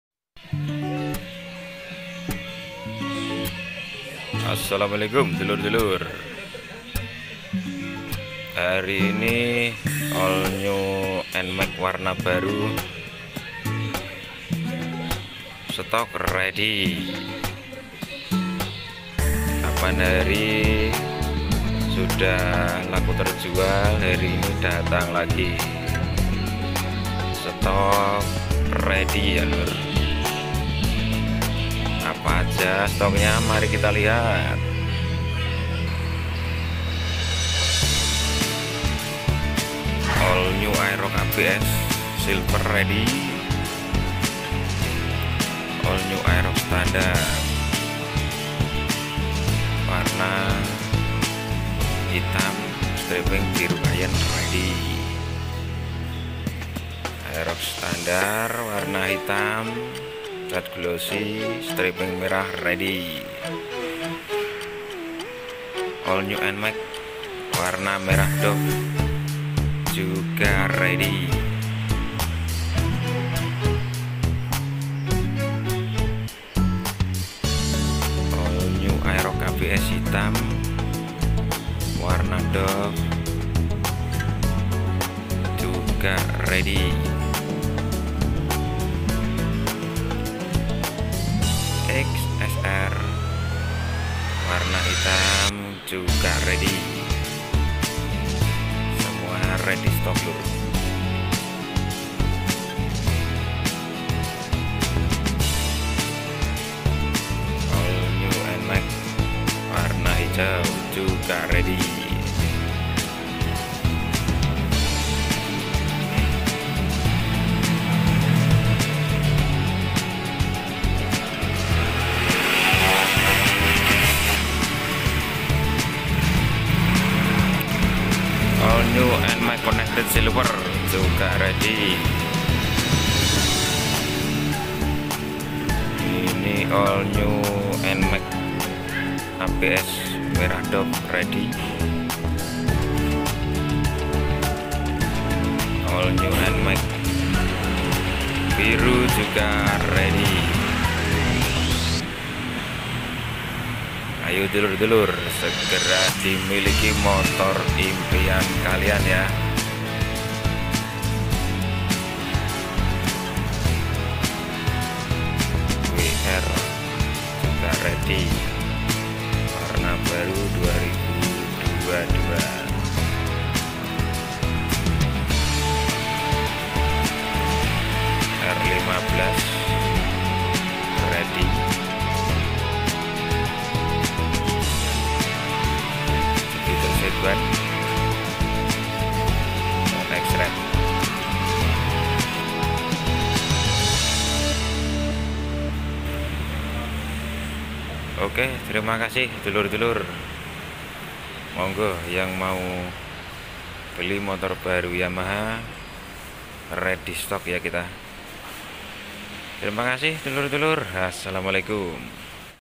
All New Nmax 2022 all varian ready stok
Assalamualaikum dulur dulur semuanya 😁🙏..  hari ini saya mau melihat kan stok all varian Yamaha yang ready stok..